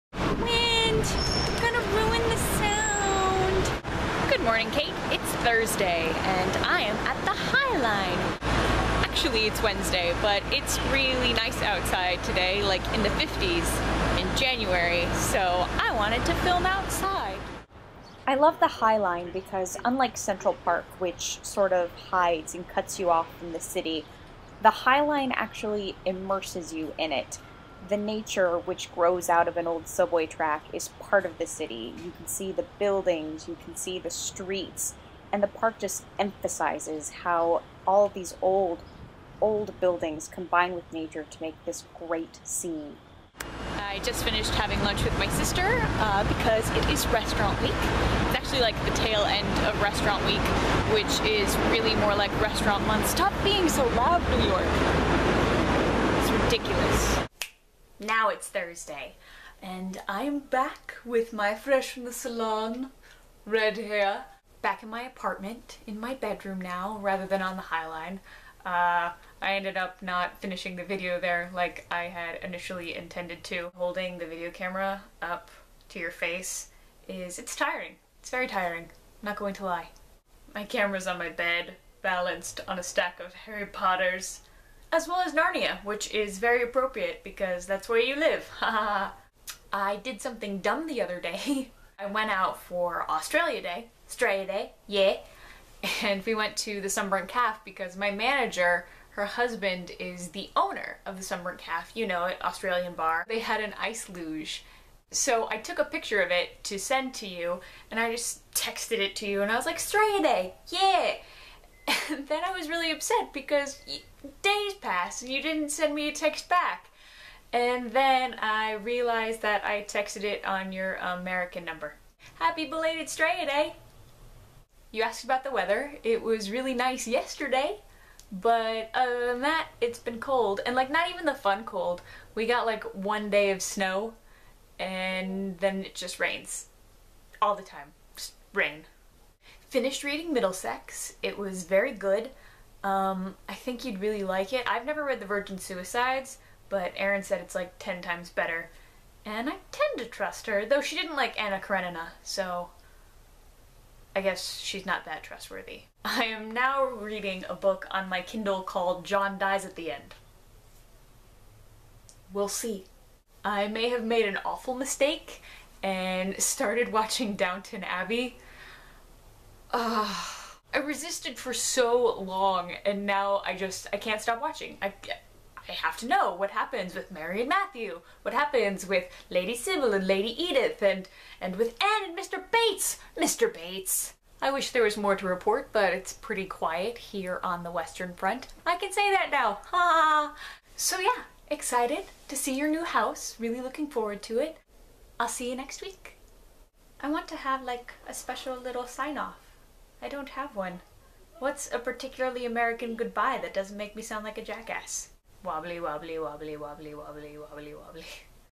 The Highline and Straya Day
Not a whole lot to do, while waiting to hear back about auditions. Maybe I'll bring you along one day... to remind you of the glamor. Until then, just chilling at parks and whatnot.